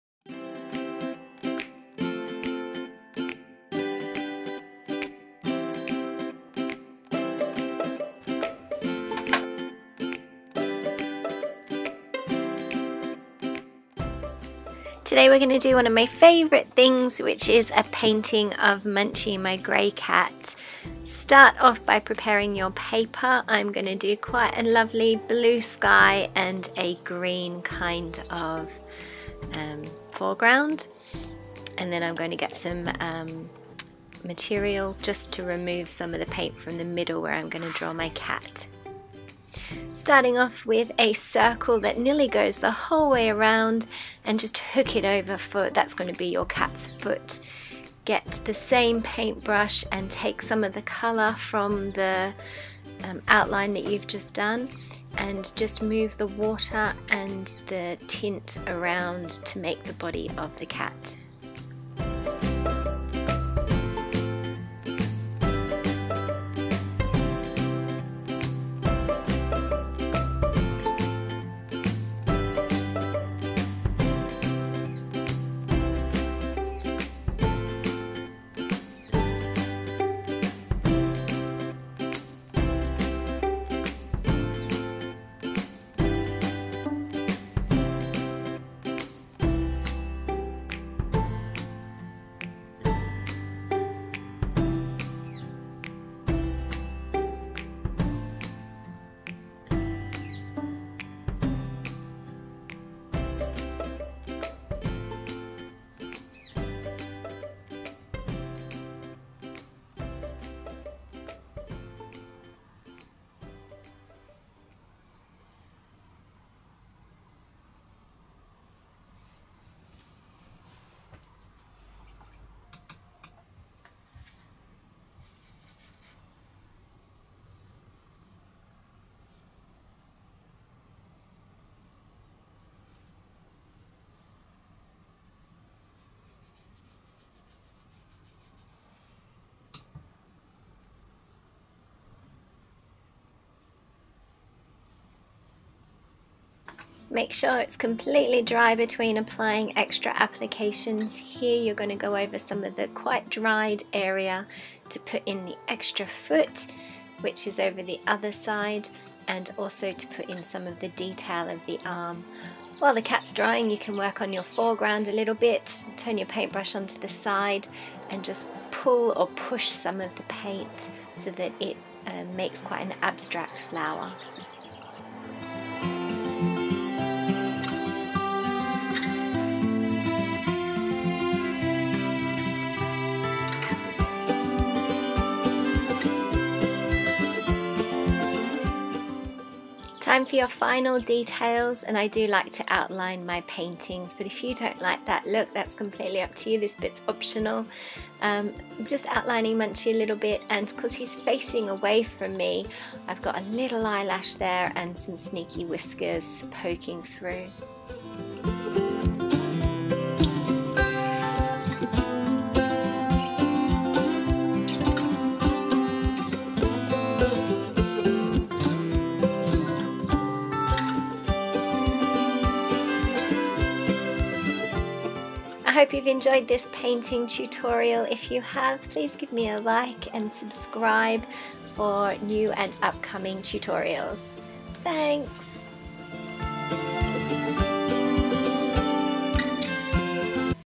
How to paint a washing cat | Watercolour tutorial |Art by Annamarie
In this video I show you how to use watercolour to paint a grey cat washing.

Please follow along and share your finished paintings with me.